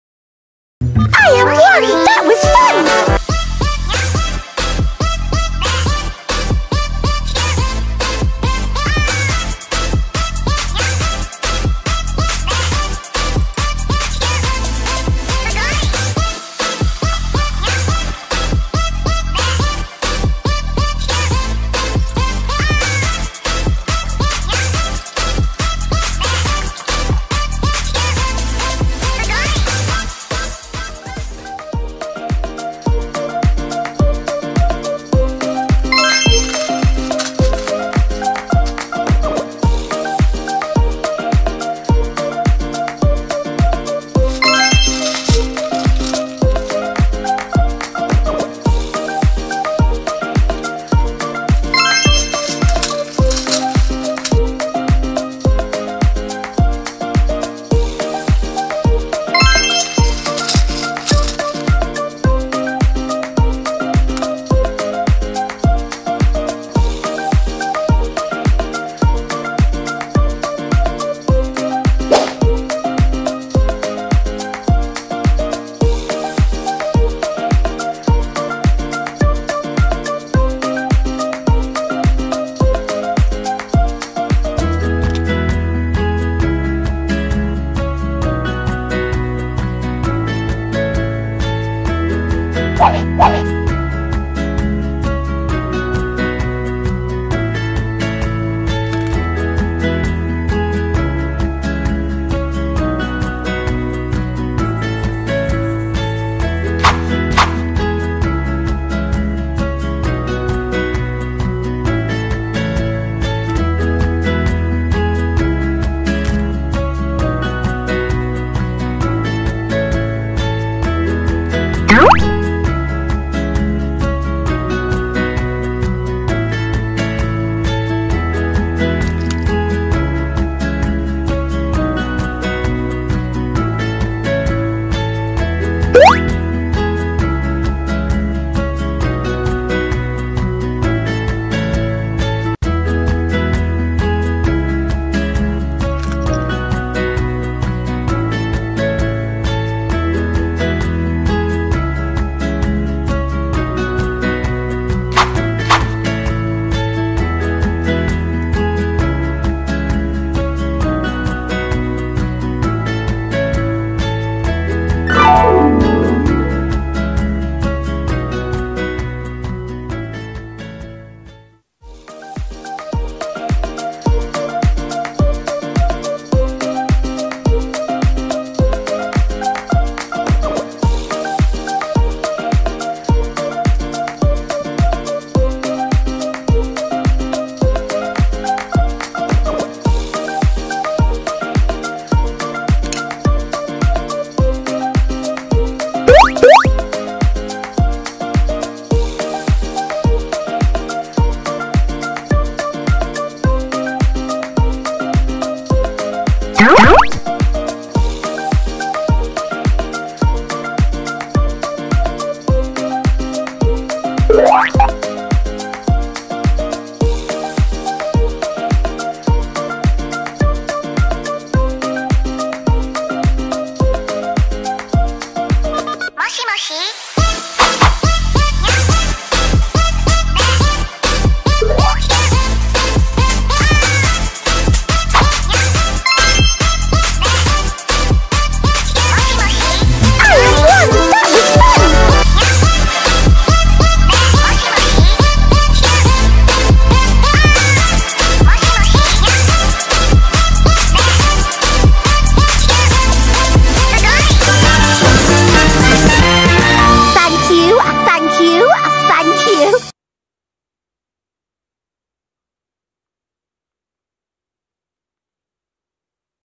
Numberblocks MAKE YOUR OWN Number One With FONDANT or Playdoh | Relaxing DIY Video
You can use either Fondant or Playdoh to make your own number blocks!